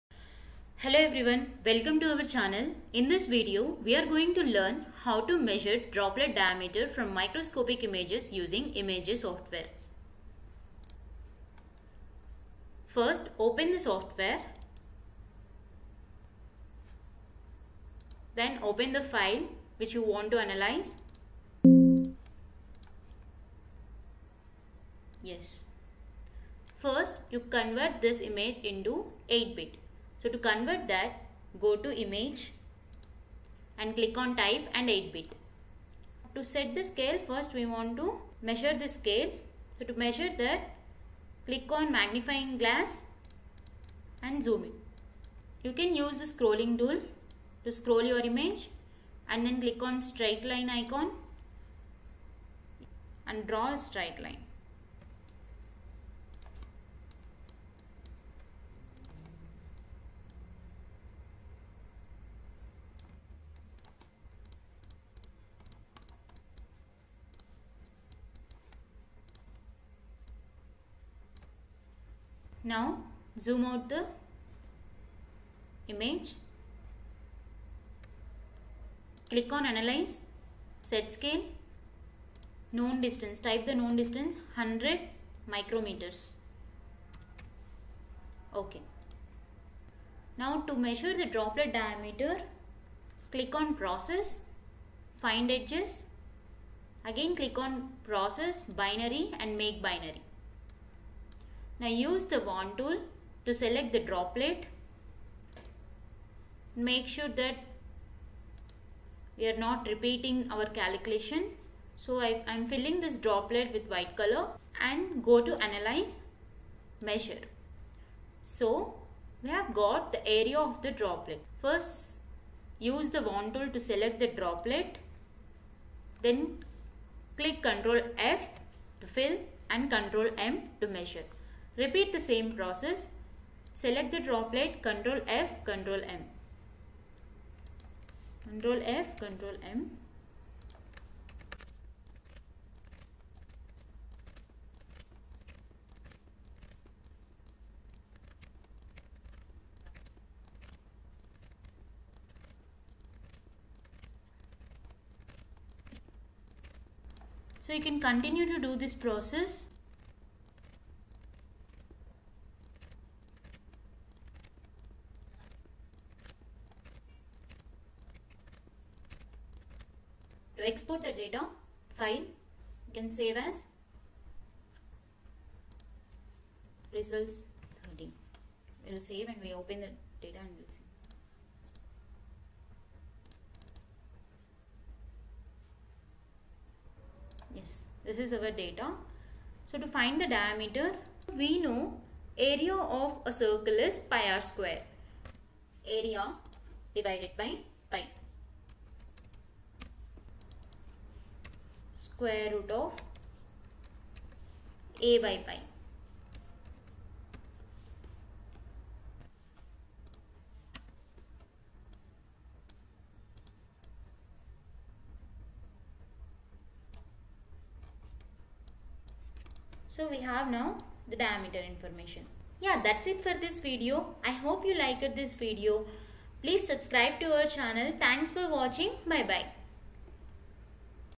MEASURE DROPLET DIAMETER USING IMAGEJ SOFTWARE| SEE HOW WAND TOOL DID WONDER😮😮😮
Hello Everyone!!!

This video is about measuring droplet diameter using the wand tool in ImageJ software.

This method makes your job easy!!!

K&M Tutors.